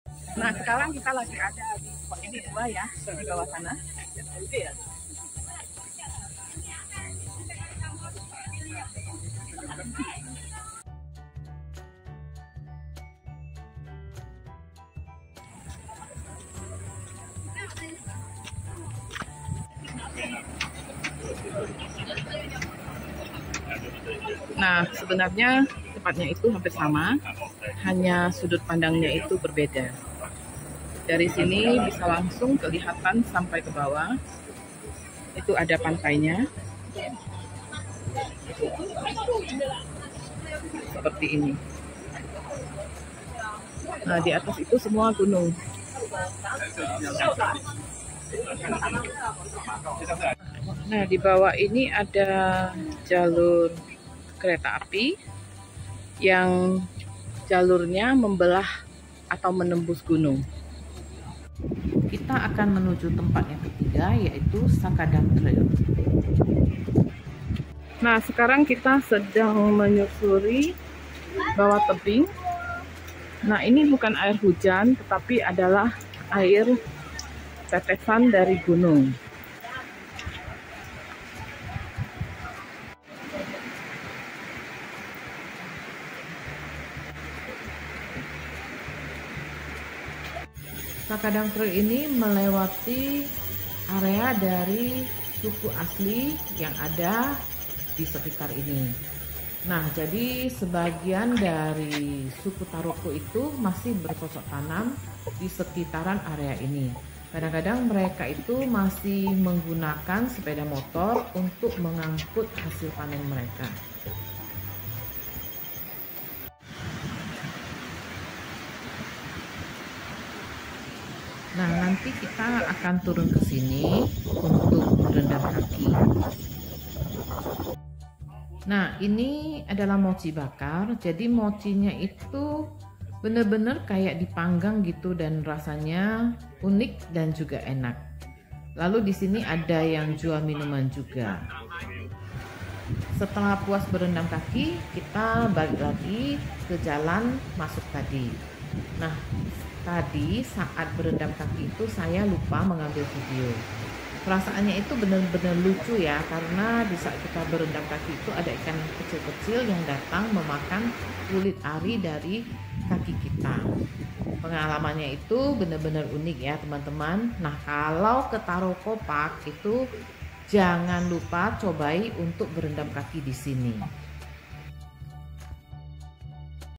TAROKO NATIONAL PARK /SHAKADANG TRAIL HUA LIEN TAIWAN /shakadangtrail
Taroko National Park Hua Lien Shakadang Trail Taiwan,disini airnya jernih sekali,jika berjalan kedalam ada yg berjualan makanan dan minuman lalu boleh berendam kaki lalu akan ada ikan2 kecil yang makan kulit mati dikaki kitamrasanya lucu banget